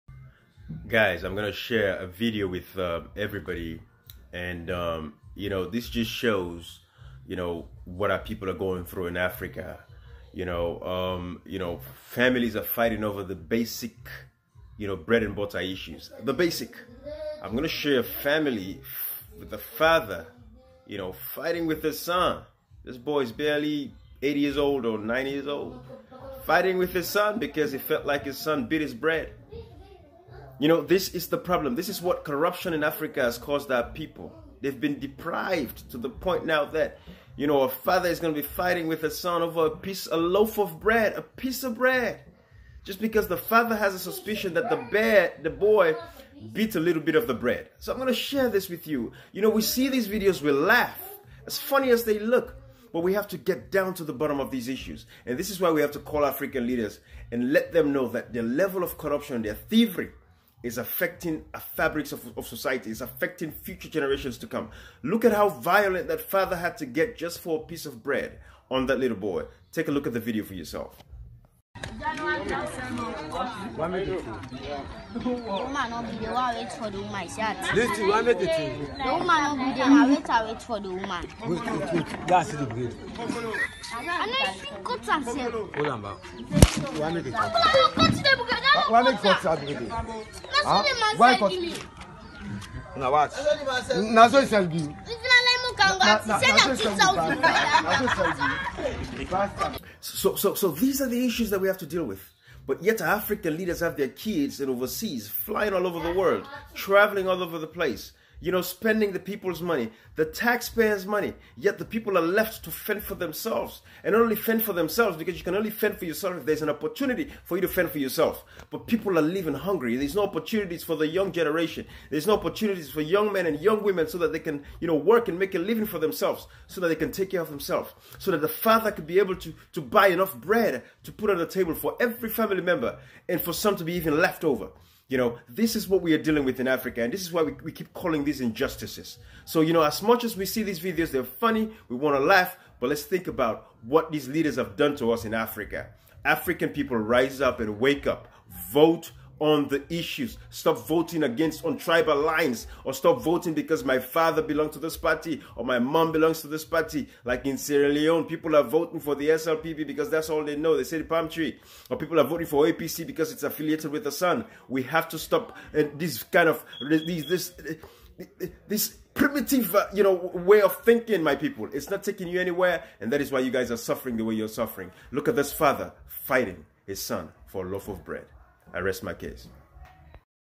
Family fighting over a loaf of bread in Africa
An uncle had to slap his nephew for allegedly biting off a piece of bread. African leaders you are destroying our peoples way of life because of corruption...African people wake up and free yourself..